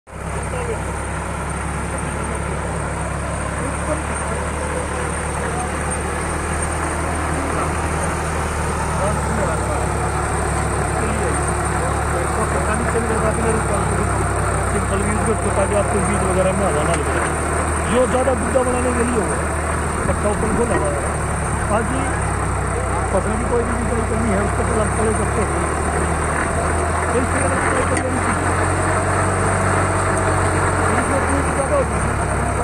फिल्ड मार्शल 813 सुपर सिडर के रोटावेटर 35 HP ट्रैक्टर का कमाल अलग अलग औजारों में तब्दील 9896782070
‎FIELD MARSHAL CO. से वीडियो